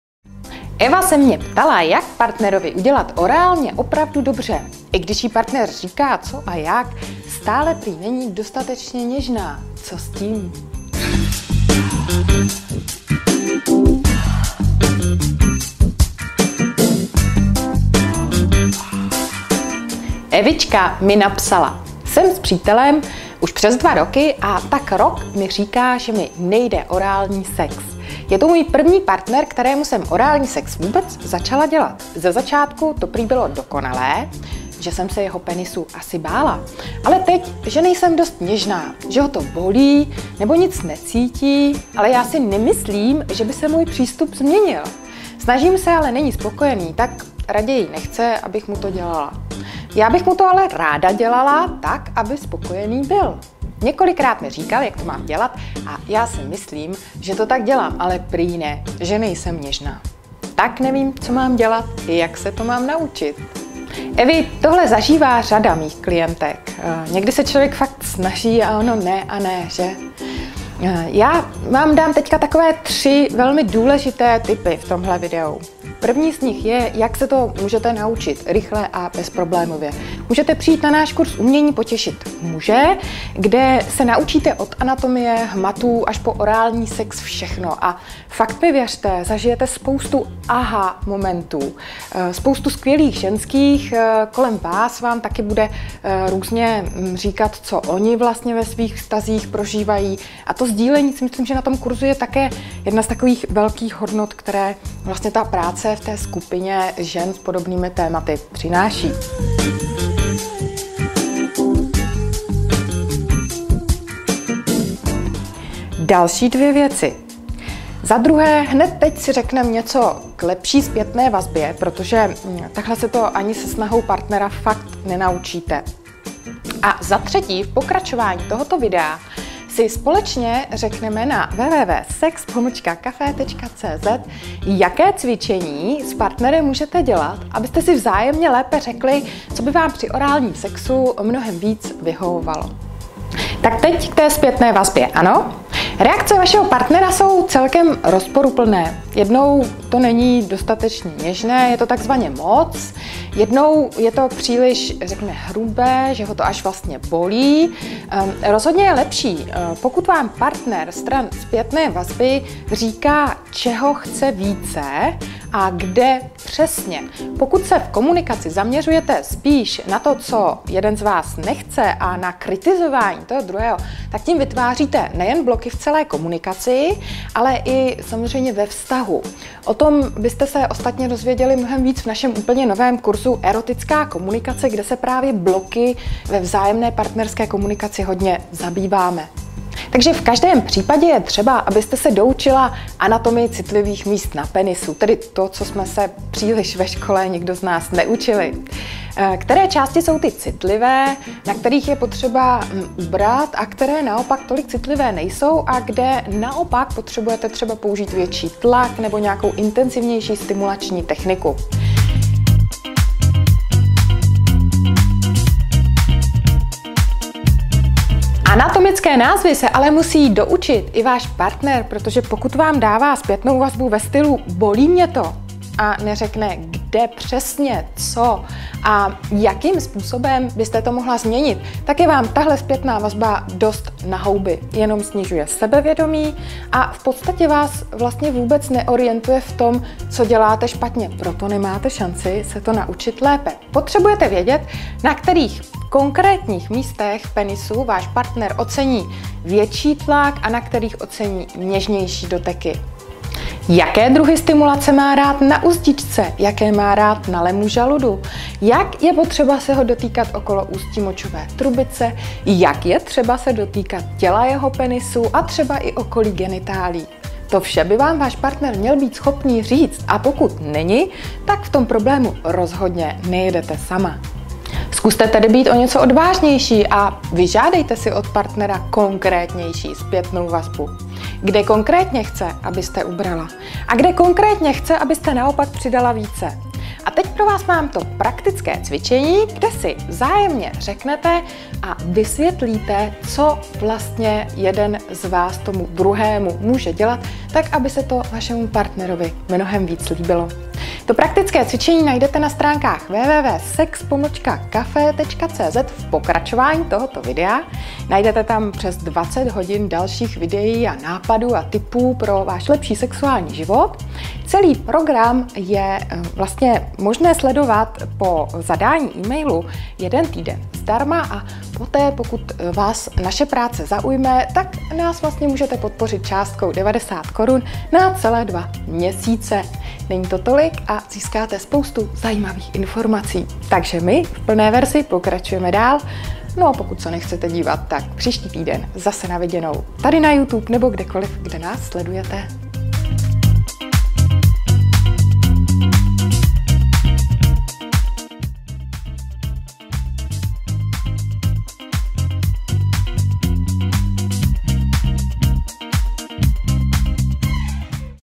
Jak na dát muži lepší felaci? - SExCAFÉ
Jak na lepší felaci, pokud si muž stěžuje, že to není ono? Možná potřebujete zlepšit komunikaci a lépe si říci, co vlastně vašemu muži dělá opravdu dobře. O tom všem v novém SExCAFÉ, kde Gaia odpovídá na dotaz Evě.